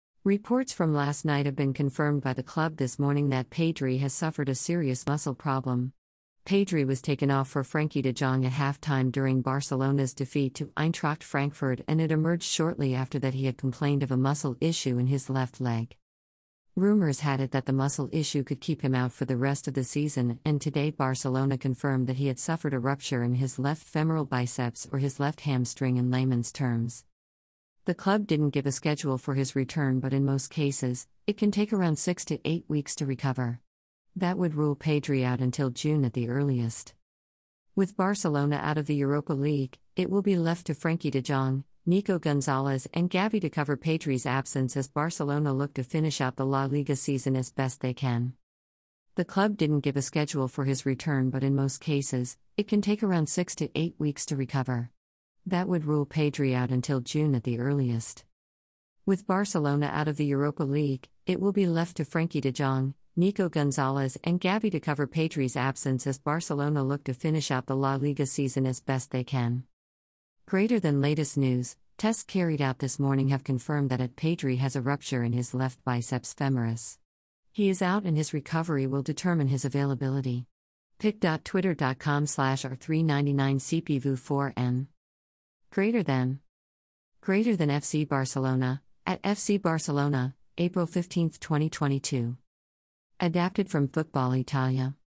Pedri injured during Barcelona and Eintracht Frankfurt game
Barcelona news